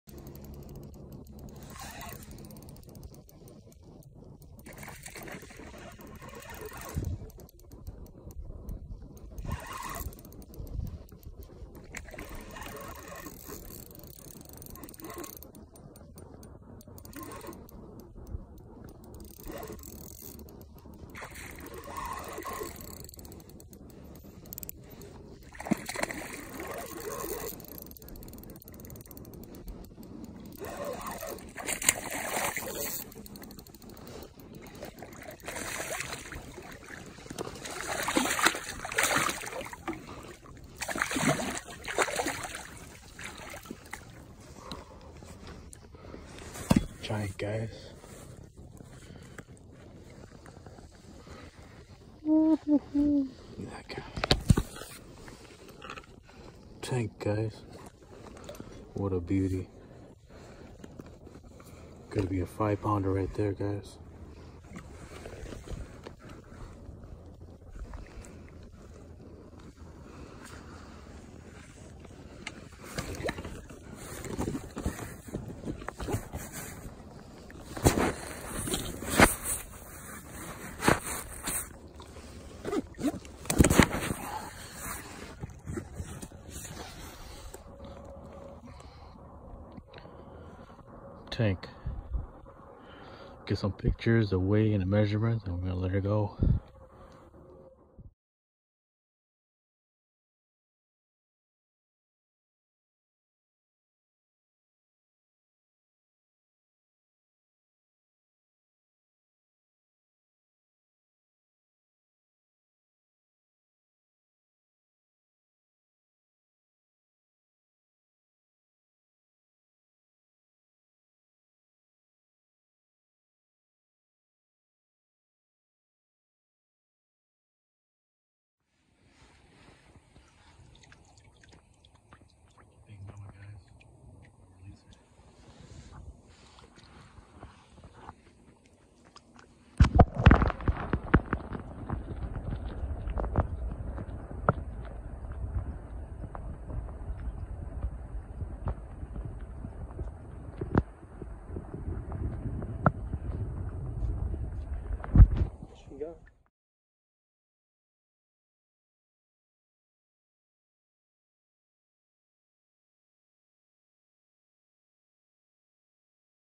Late February Smallmouth Bass
I hope everyone had a good week.  Here’s a short video of one of the bass I caught Wednesday evening.